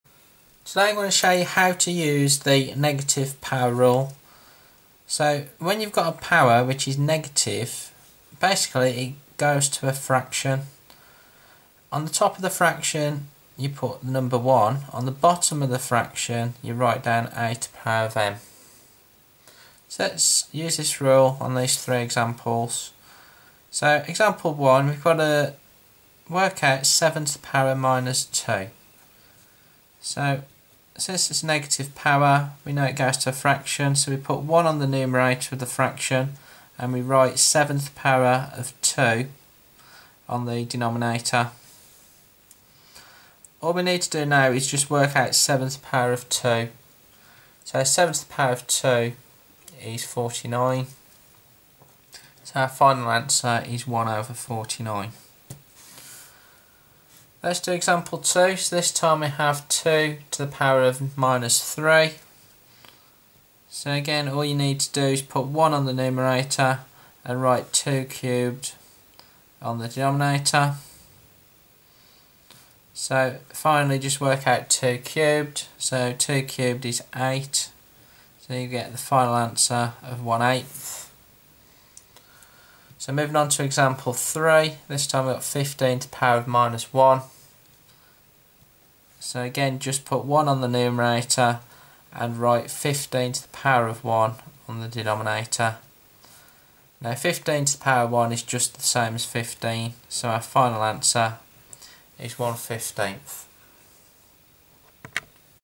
Negative Power Rule. How To Apply The Negative Power Rule To Negative Powers.
Negative powers can be turned into a fraction by putting a 1 on the numerator and a^m on the denominator. The three examples include 7^-2, 2^-3 and 15^-1. So to work out 7^-2 write 1 on the numberator and 7^2 on the denominator. 7^2 is 49 so your final answer is 1/49.